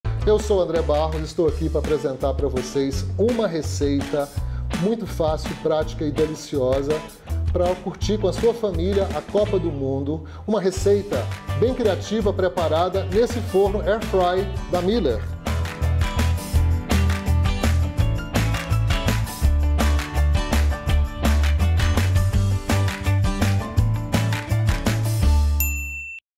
Chef André Barros | Teaser | Nosso Mundo Gastronômico | Ep.10
Receita especial pra reunir a torcida do Brasil! ✨

No episódio do Nosso Mundo Gastronômico desta semana, o Chef André Barros em parceria com a Tramontina e um super forno da Mueller, nos ensina o passo a passo de uma receita de petisco que é rápida, saborosa e super versátil para reunir a família e os amigos.

👉🏻Costelinha suína ao molho barbecue e mandioquinha.

⏰ Quer saber mais? Acompanha a receita completa em nosso feed do Instagram e aqui no nosso canal do Youtube.

🔔 Agradecimentos:
✓ Chef André Barros
✓ Tramontina
✓ Mueller
✓ Philco